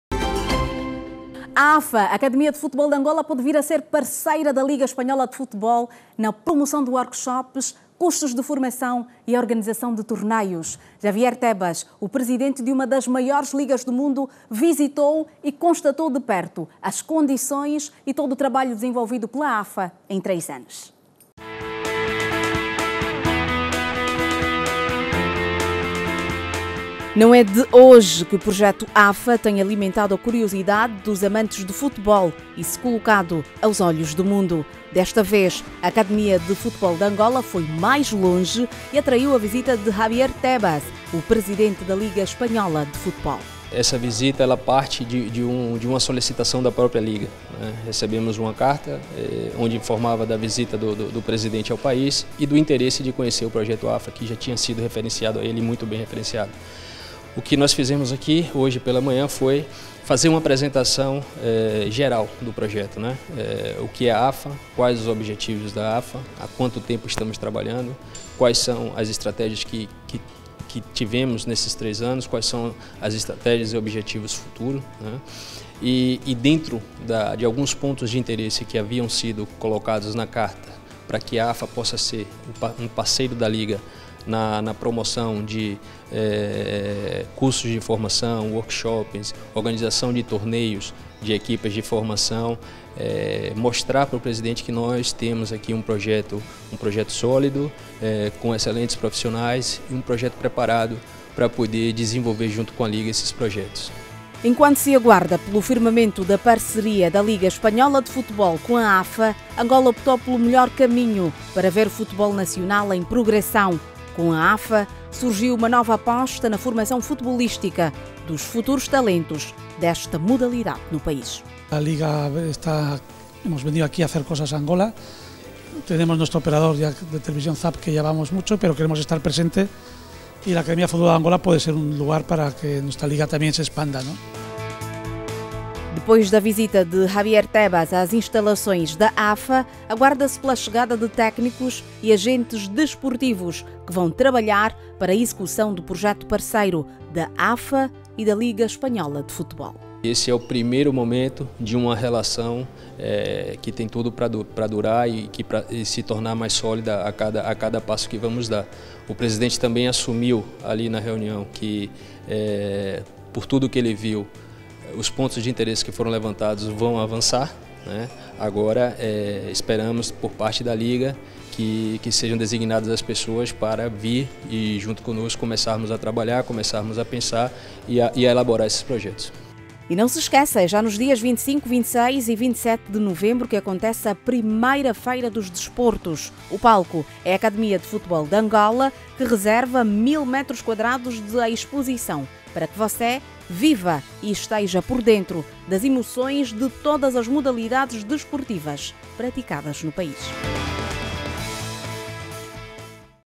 Angola Magazine | AFA atrai presidente da liga espanhola
A Academia de Futebol de Angola (AFA) está a somar pontos e desta vez recebeu a visita do presidente da liga espanhola de futebol, Javier Tebas.